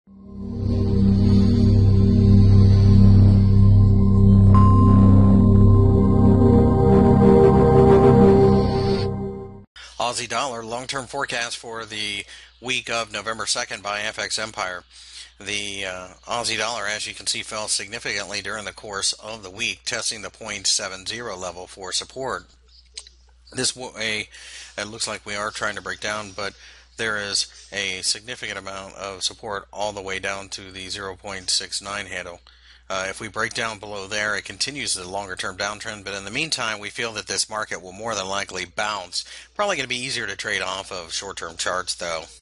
AUD/USD Forecast for the week of November 2, 2015, Technical Analysis
November 2, 2015 currency weekly technical analysis for the AUD/USD pair.Find more information about Forex News Forex Technical Analysis Forex Fundamental Analysis and Forex Brokers on FX Empire